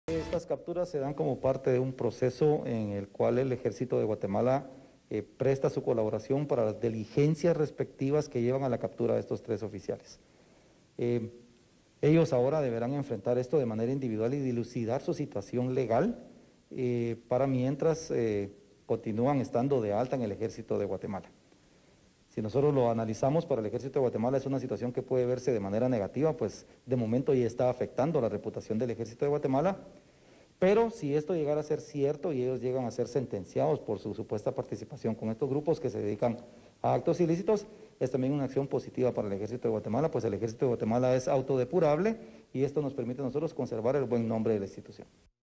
Ministerio de la Defensa se pronunció sobre detención de tres militares
Por la presunta vinculación de los elementos militares con el narcotráfico

El Ministerio de la Defensa Nacional se pronunció sobre la detención de tres militares, por su presunta vinculación con el narcotráfico.